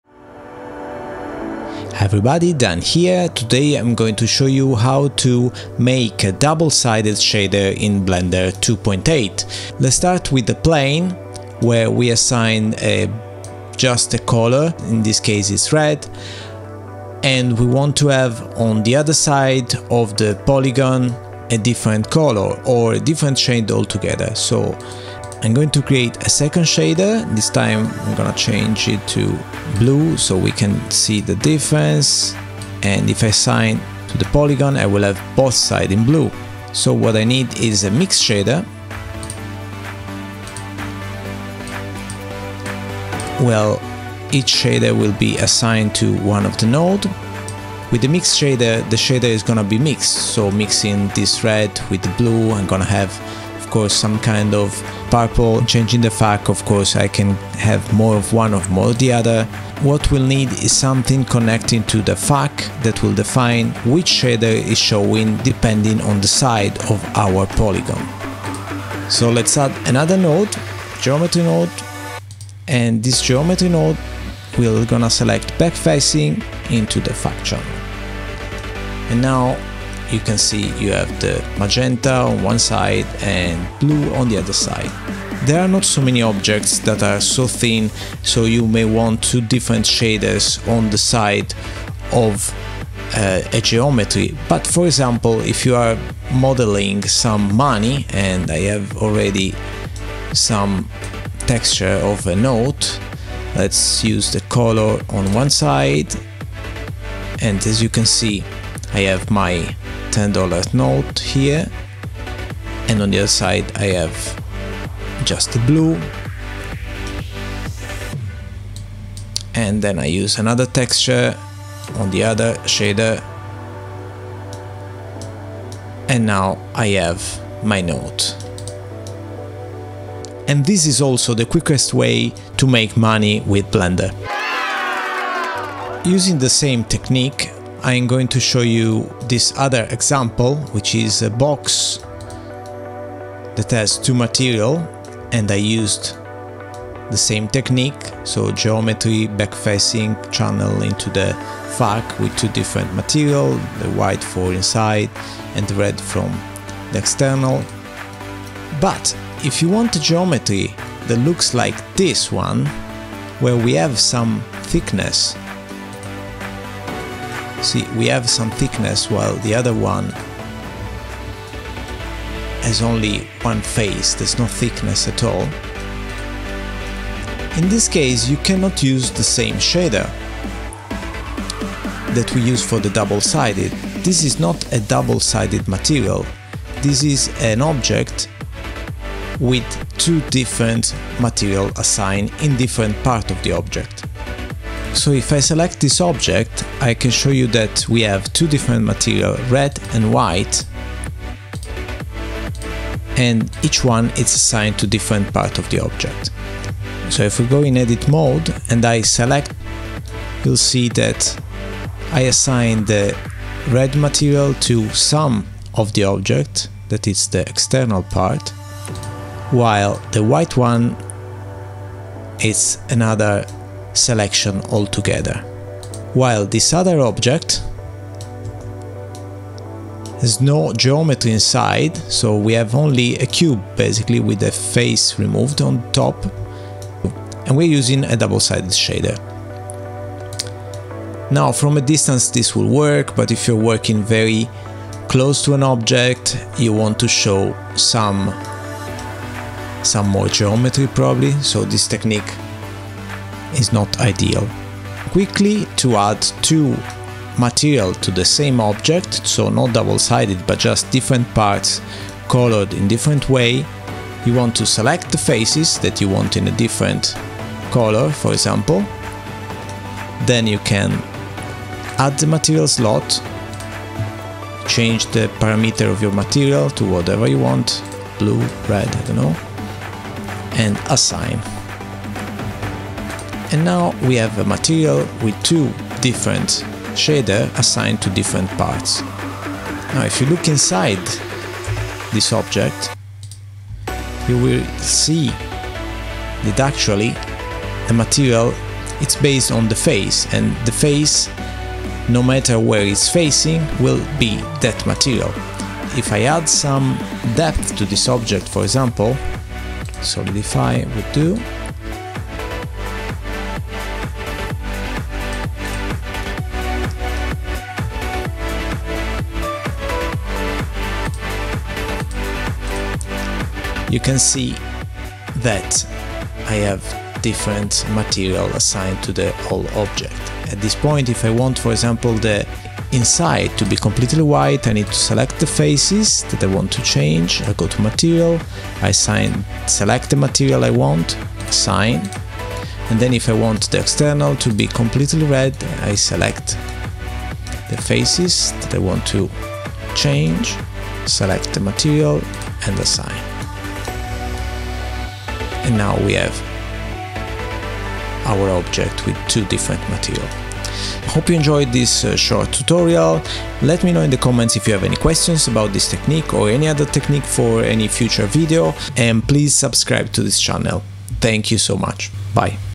How to make a two-sided shader material in Blender 2.8 EEVEE and Cycles - Beginner Tutorial
The quickest way to make a two-sided shader material in Blender 2.8, the same technique works for Cycles and EEVEE as well. As a bonus, I also compare and explain how to assign multiple materials to a mesh!

- Yes the note UV has not been edited and they are mirrored, maybe a future video will talk about how to UV map texture in Blender!

- Yes, there's a daddy joke

- Yes, you guys are superstars! Thanks so much for following me!